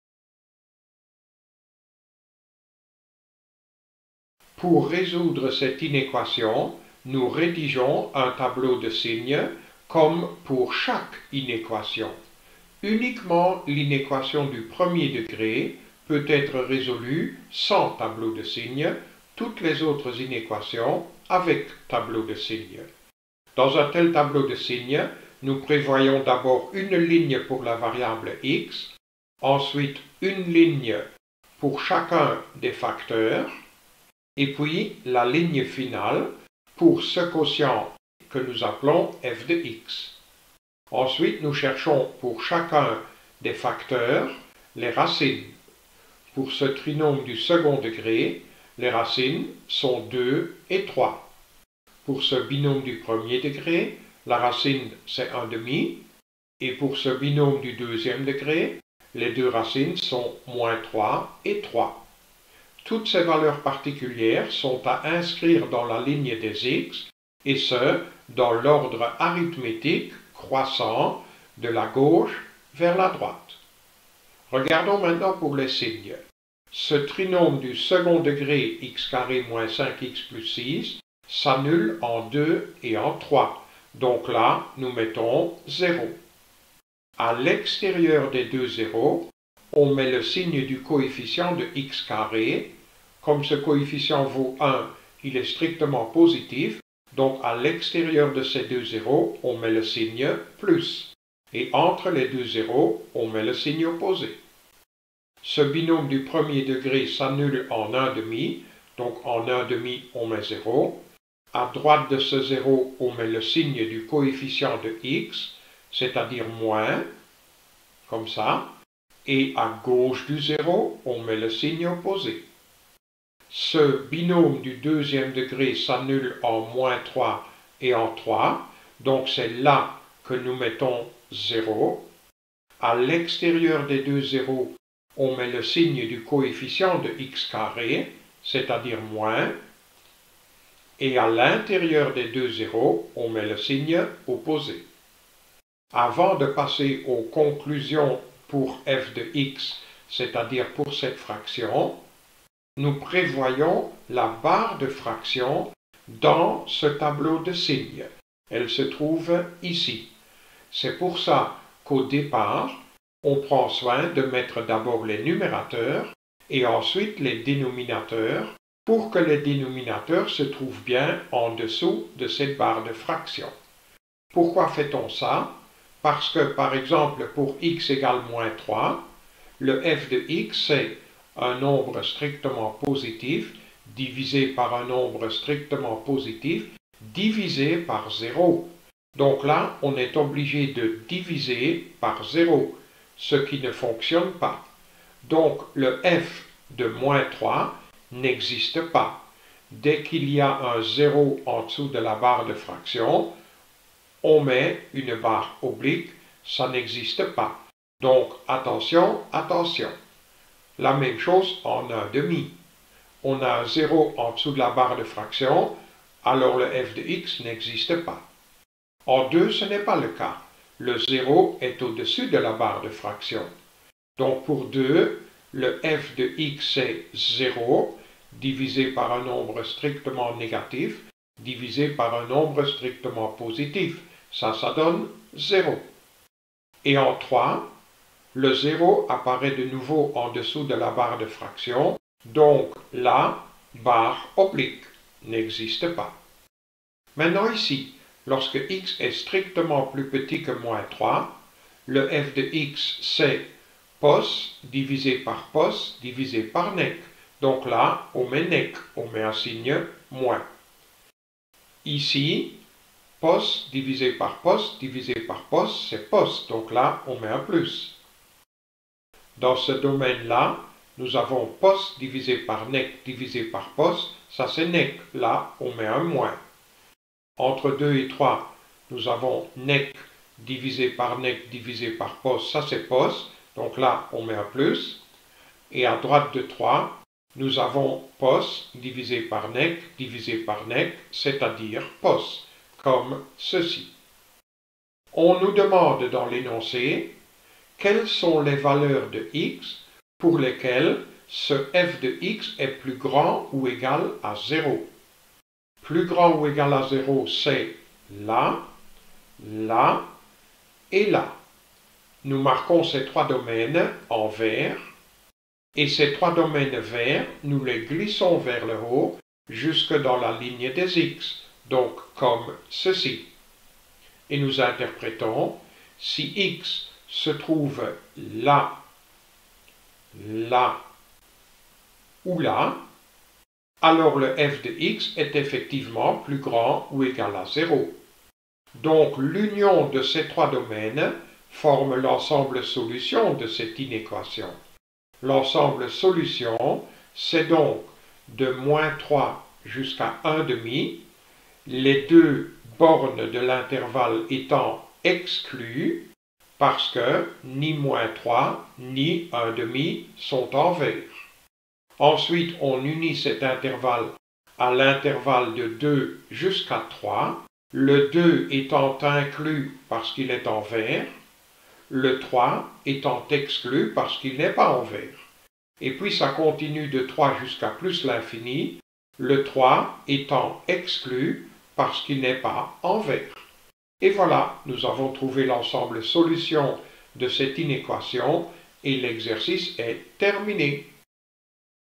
Inéquations 014
Nous résolvons une inéquation à une inconnue au moyen d'un tableau de signes.